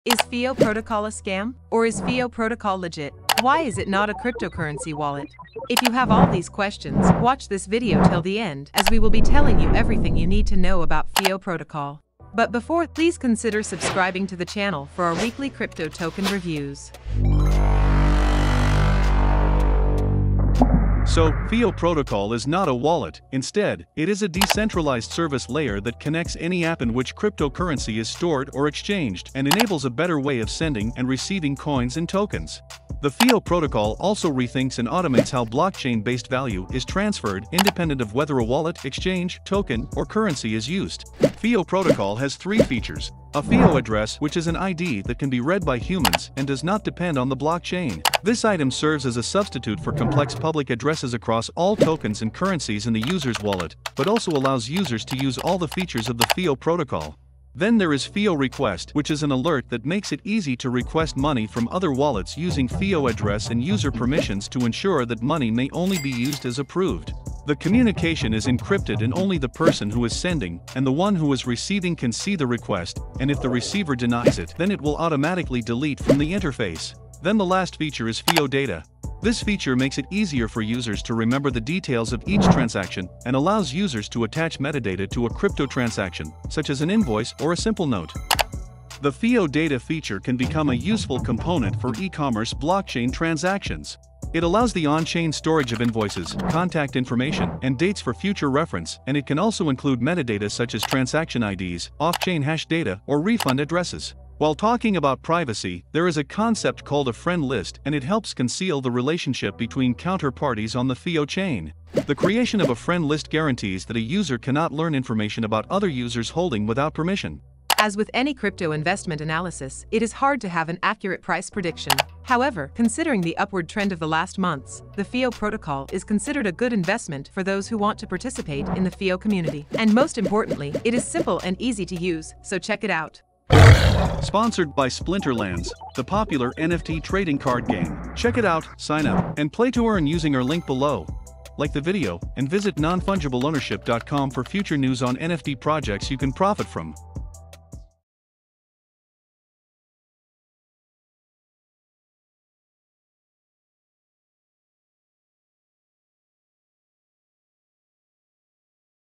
The FIO Protocol and how you should benefit! - FIO token
The FIO protocol helps blockchain transactions to be easily send and received!

What is your price prediction for the FIO token and the FIO protocol?

Timestamps:
0:00 What is the FIO protocol
0:40 The three features of FIO
1:57 Is FIO a good investment